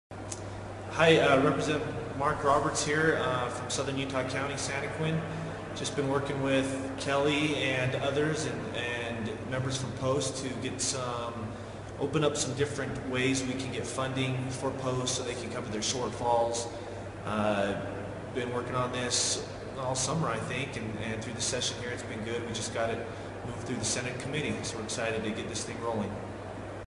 Rep Marc Roberts talks about FOP & POST funding.
Shout out to FOP on POST funding